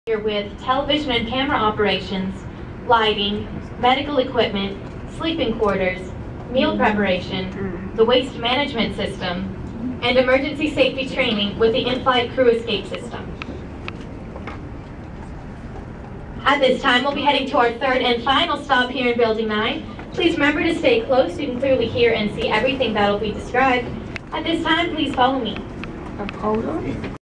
NASA, Johnson Space Center visitor viewing level.
A short movie showing the visitor viewing level on NASA Johnson Space Center, Houston, Texas overlooking the ESS, MIR  and space shuttle training center 2011.